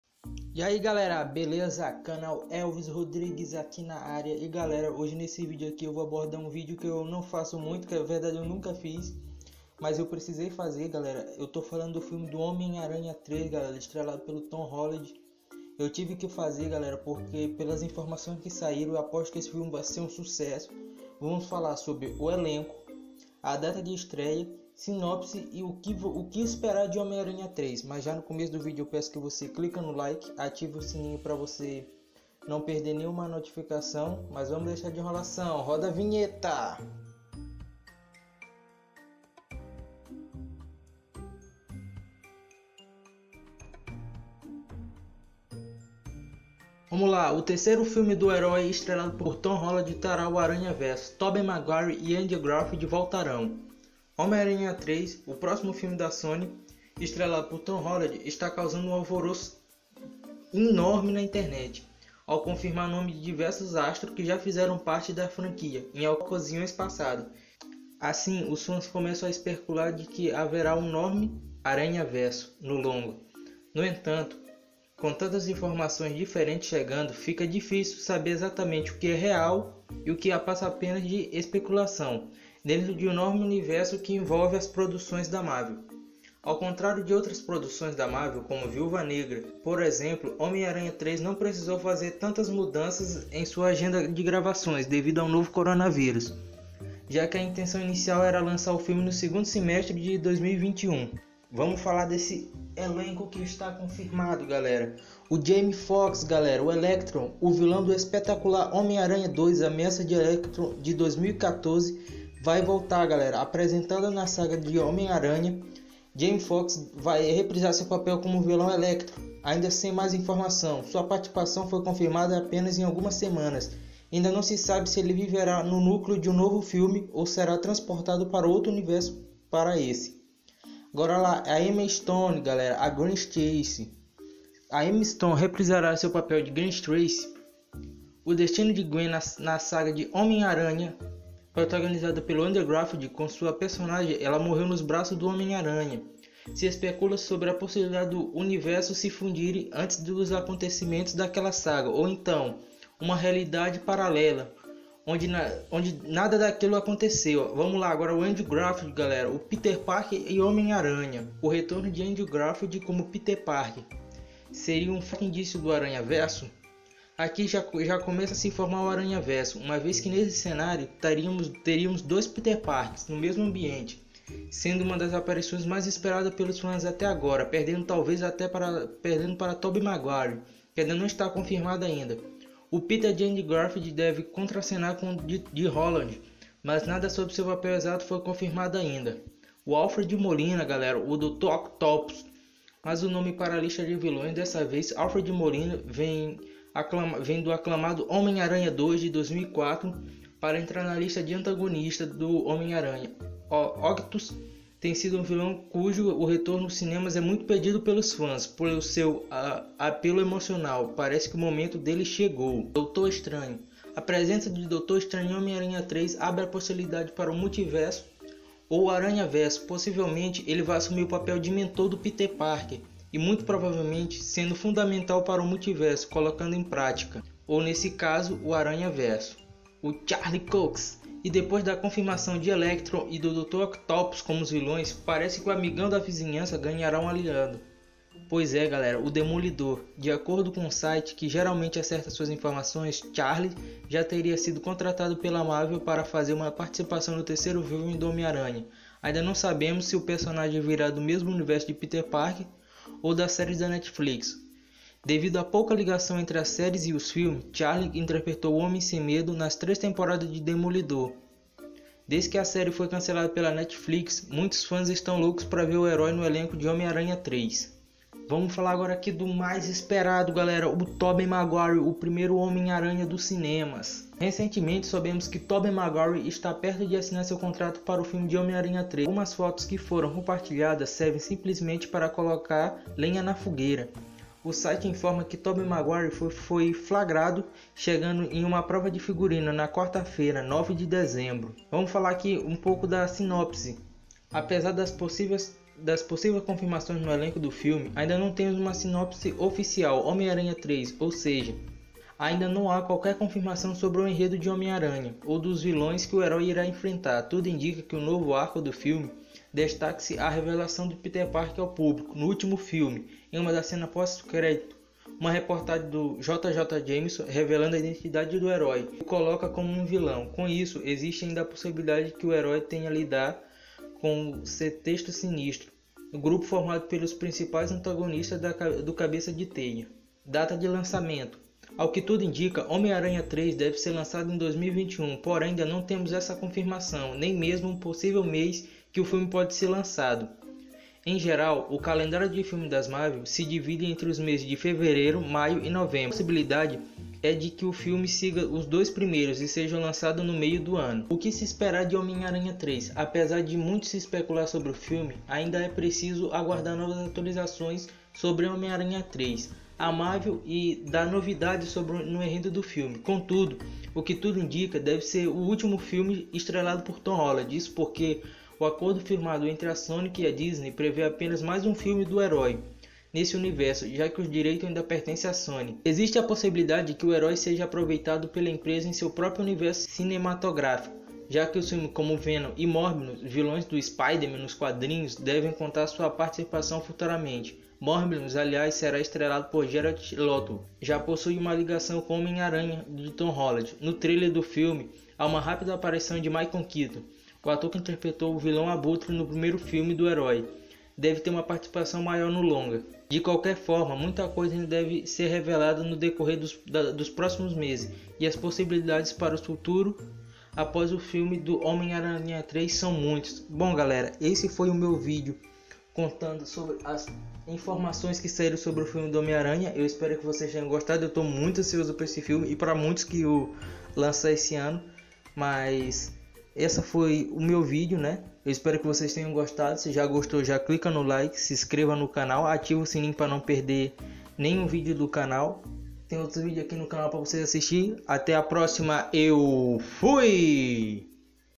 HOMEM ARANHA 3! Tobey Maguire e Andrew Garfield vão aparecer no filme e muito mais!
Homem-Aranha 3 sem sombra de duvida um dos filmes mais esperados pelos fãs da  Marvel o ator que apareceu no filme Homem-Aranha 3 de Tobey Maguire esta sento cogitado para ter retorna em Homem-Aranha 3 do UCM.

E pelo o que parece as novidades do Multiverso da Marvel não para por ai e a Sony revela que gostaria de ver o Venom de Tom Hard e o Deadpool de Ryan Reynolds juntos em um de seus filmes!